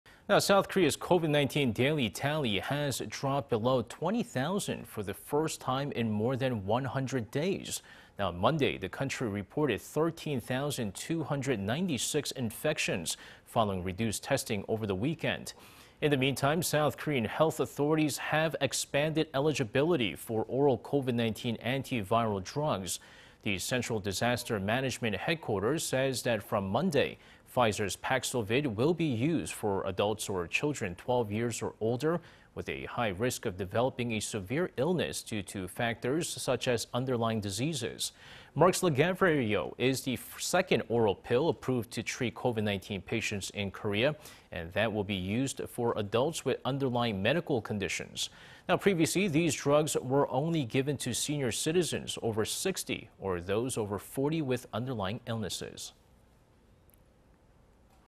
S. Korea reports 13,296 new cases of COVID-19 on Monday; Paxlovid available for those aged 12 or ...
S. Korea reports 13,296 new cases of COVID-19 on Monday; Paxlovid available for those aged 12 or above

오늘부터 팍스로비드 처방대상 12세 이상 기저질환자로 확대

South Korea's COVID-19 daily tally has dropped below 20-thousand for the first time in more than 100 days.
On Monday... the country reported 13-thousand-two-hundred-96 infections following reduced testing over the weekend.
In the meantime... South Korean health authorities have expanded eligibility for oral COVID-19 antiviral drugs.
The Central Disaster Management Headquarters says that, from Monday, Pfizer's Paxlovid will be used for adults or children 12 years or older with a high risk of developing a severe illness due to factors, such as underlying diseases.
Merck's Lagevrio, is the second oral pill approved to treat COVID-19 patients in Korea,... and will be used for adults with underlying medical conditions.
Previously, these drugs were only given to senior citizens over 60,... or those over 40 with underlying illnesses.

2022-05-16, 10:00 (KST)